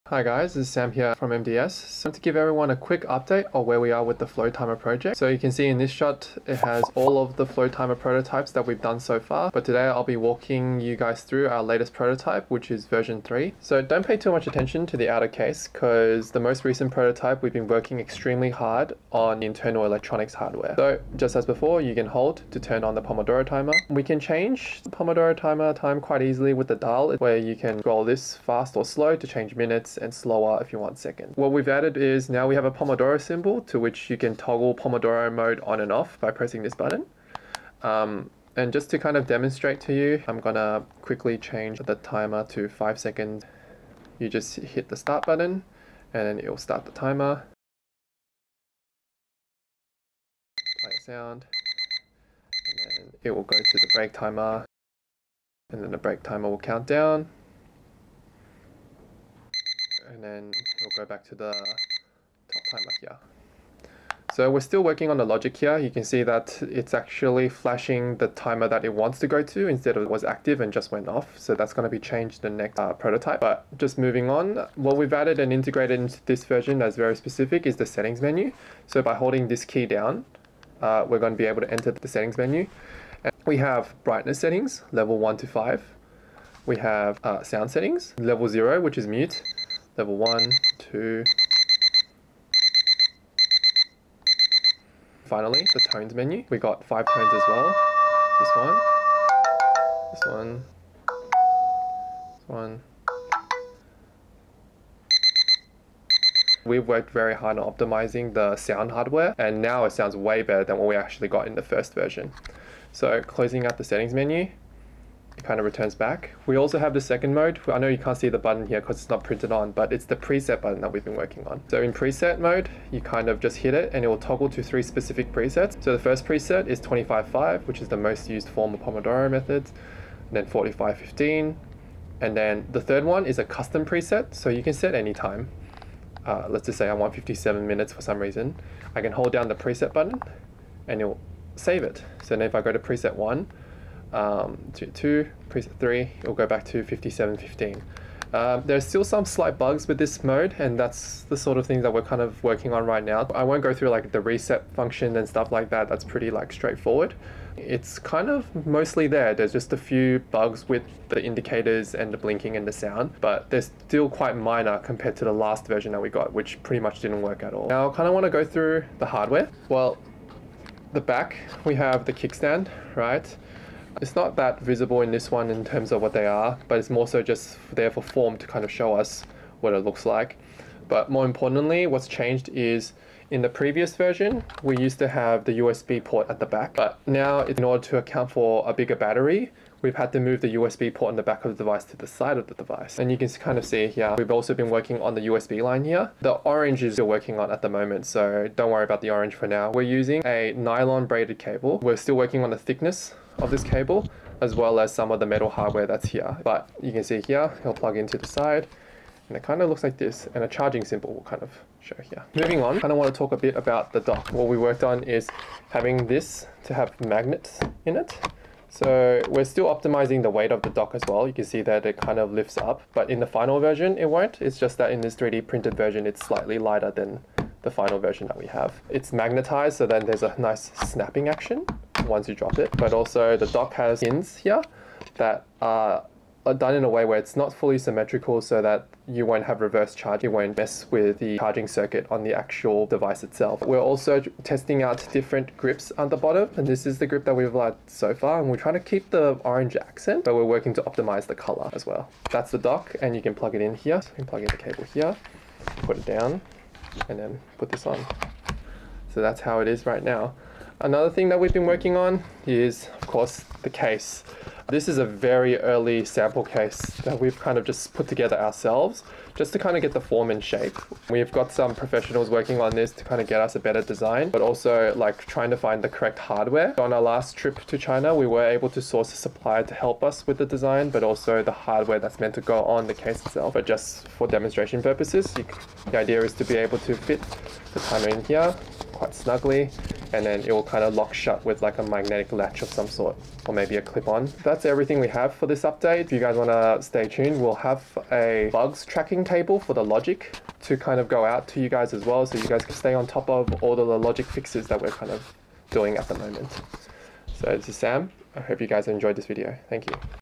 Flow Timer - Prototype v3 - Update Video
We’re almost there!

The Flow Timer is entering its final stages before launch! Battery tests are underway, software is nearly polished, and V4 hardware is on the horizon.

🔋 Battery Upgrade - Now 1500mAh (50% more!)
🔊 Audio Overhaul - 16-bit DAC + larger speaker incoming
🧠 Software - Core logic is solid, just minor tweaks left
📦 Timeline - V4 prototype drops mid-June, Kickstarter launch in July!

Thanks for sticking with us – the finish line is in sight 🎯